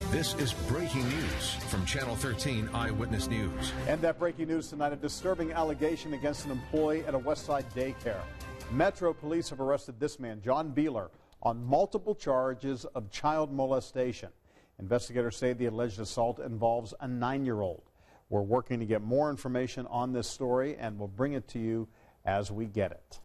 Daycare worker arrested on charges of child molestation
John Beeler faces multiple charges of child molestation. Police say the incident may involve a 9-year-old.